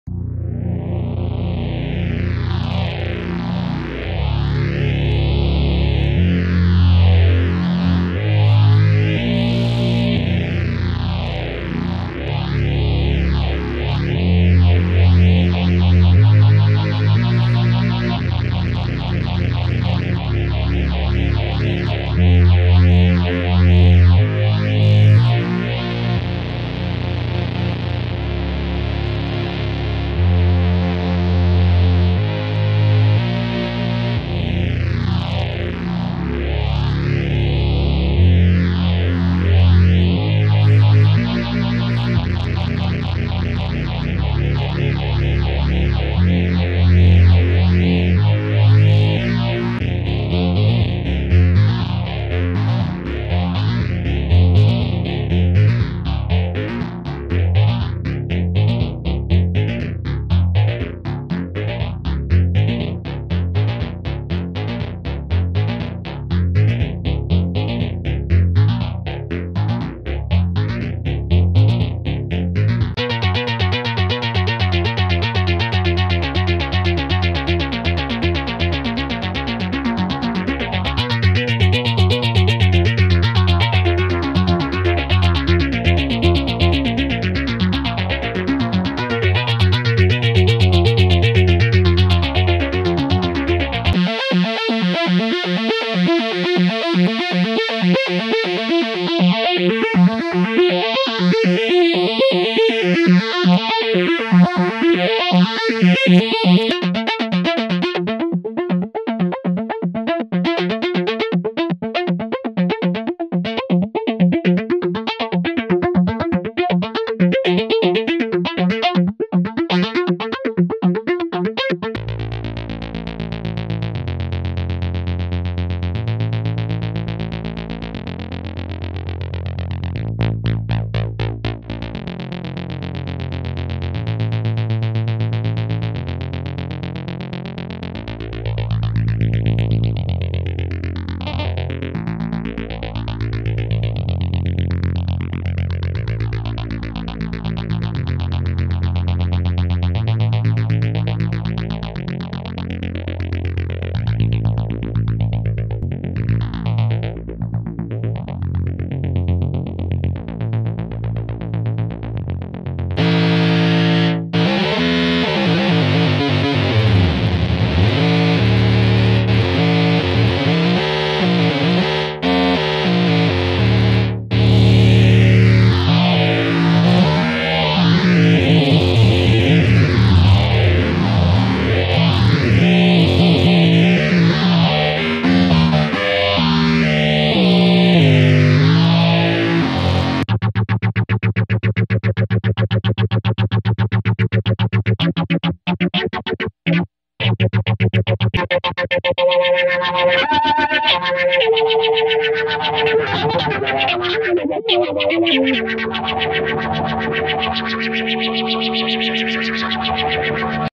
Maestro Oberheim Phase Shifter Pedal Demo w/ A Synthesizer #4
Demo of the Maestro PS-1B Phase Shifter designed by Tom Oberheim.  This is the rarer version with the Variable Speed knob.  The knob does exactly what the switches do, with the ability to set the speed anywhere in between.  The knob only works when the slow phase switch is set.  As the switches OR knob settings are changed you will notice a smooth and gradual transition in speed.

The synthesizer is the Korg reissue ARP Odyssey.  If you're interested in the patches hopefully the footage shows the patch settings to some degree.  I added a small amount of delay for stereo widening.  During one section I use the Moroder delay trick to change simple 8th notes to stereo-panned 16-th notes, and then apply the phaser.   I was recording levels pretty hot, sorry there is some clipping in parts of the audio.

All audio and video by G-Storm Electro c.2016